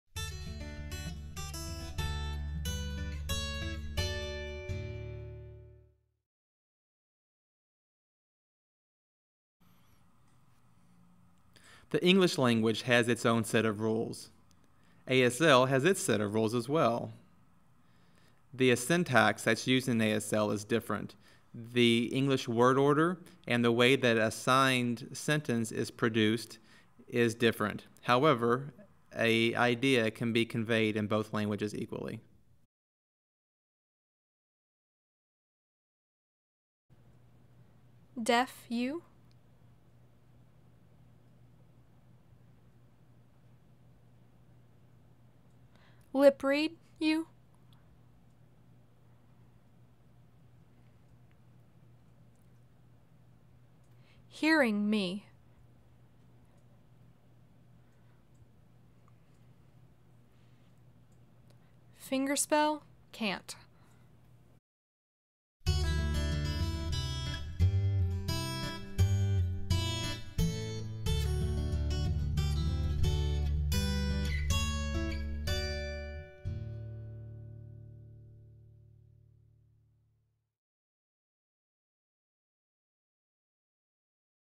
90 Signs in 90 Days #87 (Bridges for Deaf and Hard of Hearing)
We continue looking at work order and sentences on Day 87 of '90 Signs in 90 Days.'  For more, please visit bridgesfordeafandhh.org.